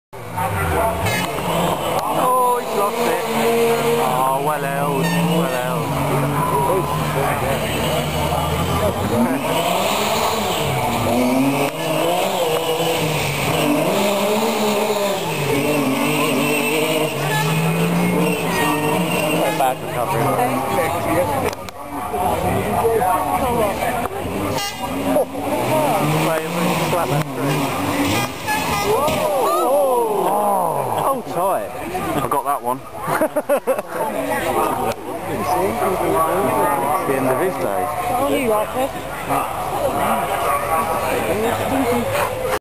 Porsche crash at the Chatsworth House Rally Show
Porsche gets it a bit wrong going through the water splash at the Rally Show held at Chatsworth House